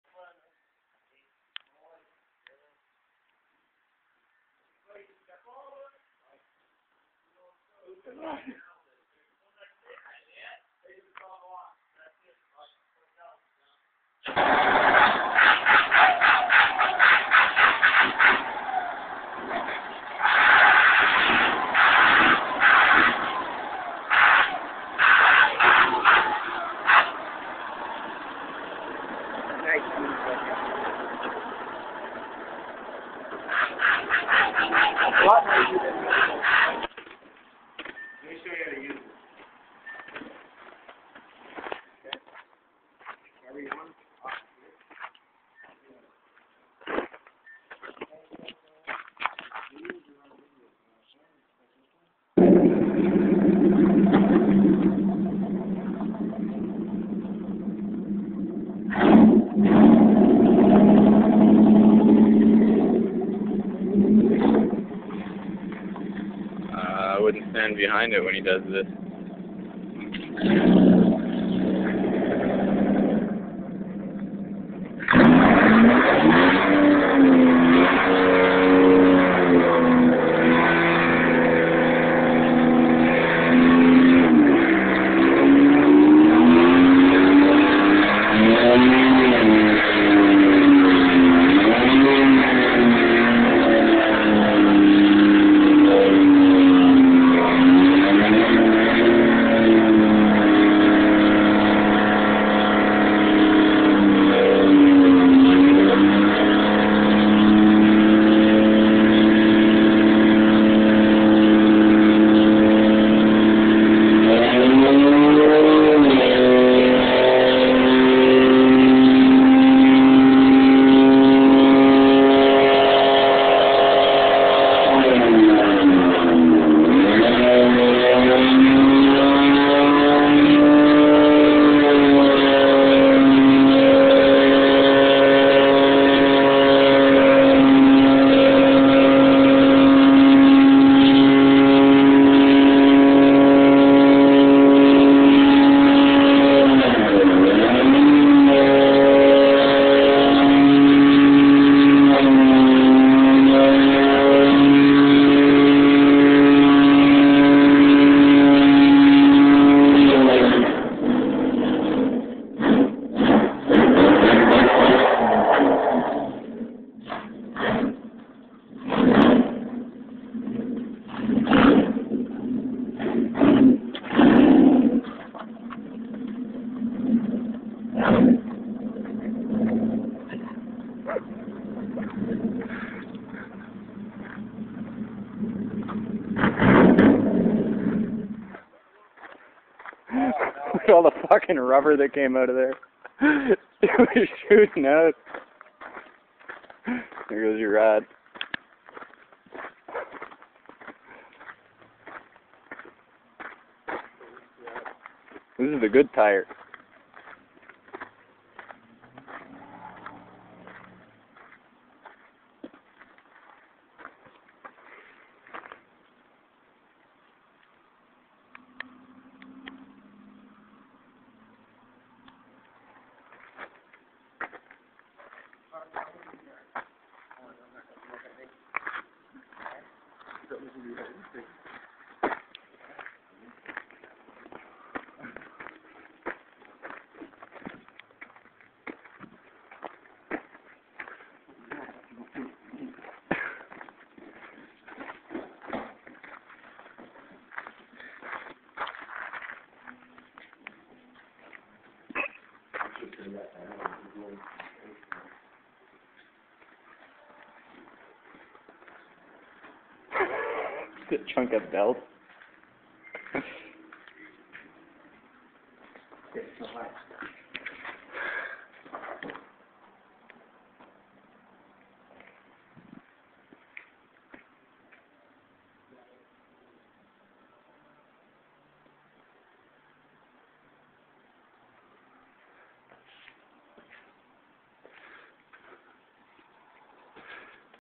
Junk Yard Marc with another 929 doing burnouts 2
Later the same day Marc takes the 929 back to his place and blows the tires of on the cement slab.